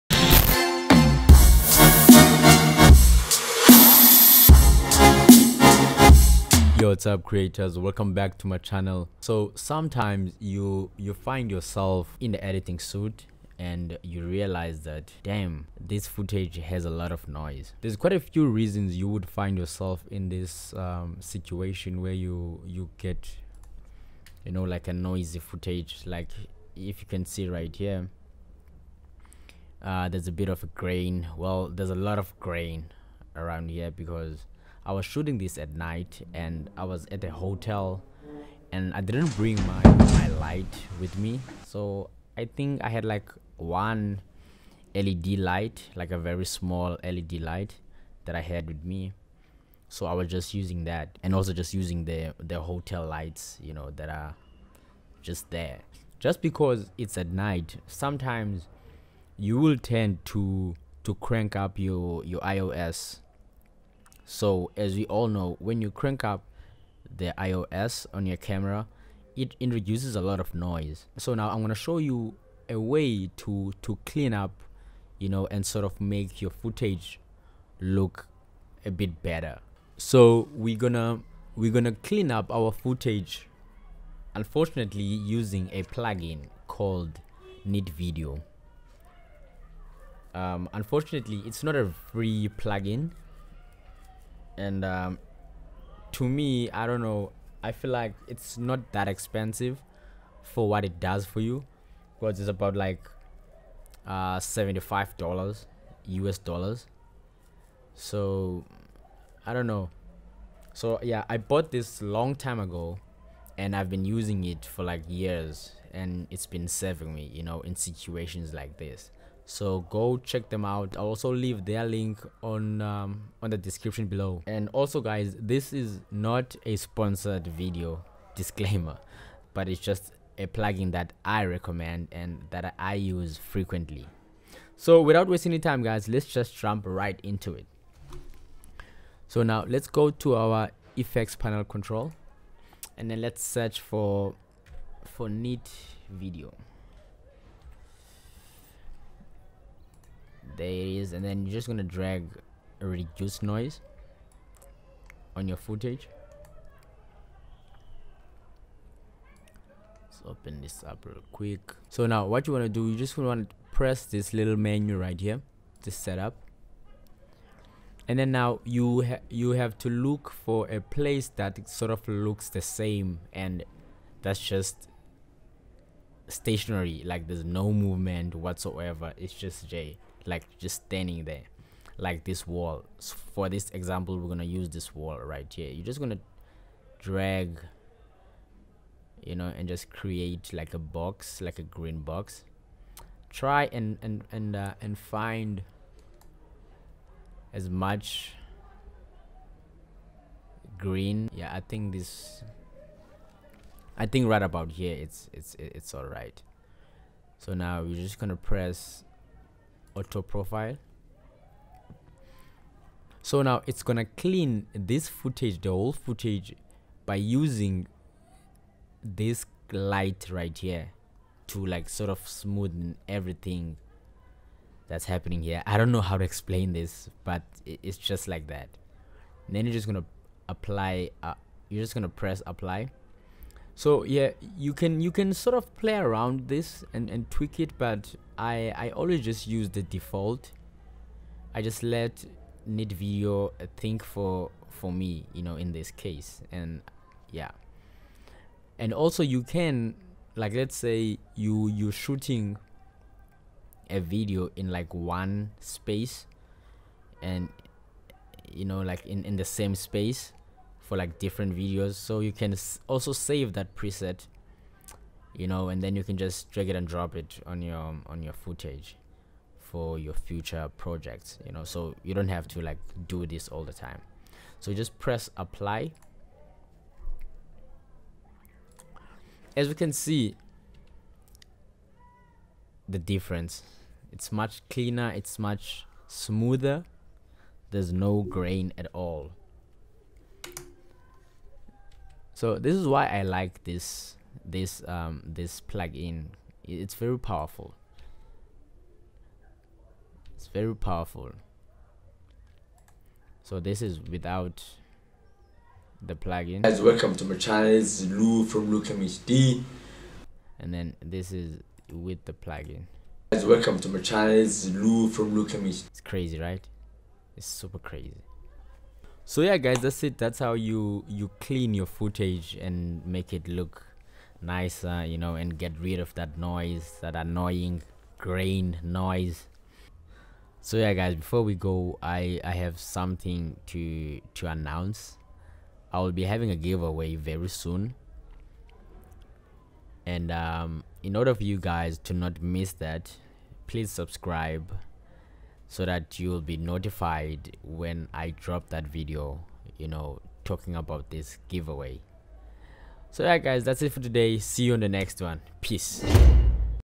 HOW TO REDUCE NOISE IN YOUR FOOTAGE
HOW TO REDUCE NOISE IN YOUR FOOTAGE II

HEY GUYS TODAY I WILL TEACH YOU HOW TO USE A PLUG-IN CALLED NEAT VIDEO TO REDUCE NOISE IN YOUR CLIPS.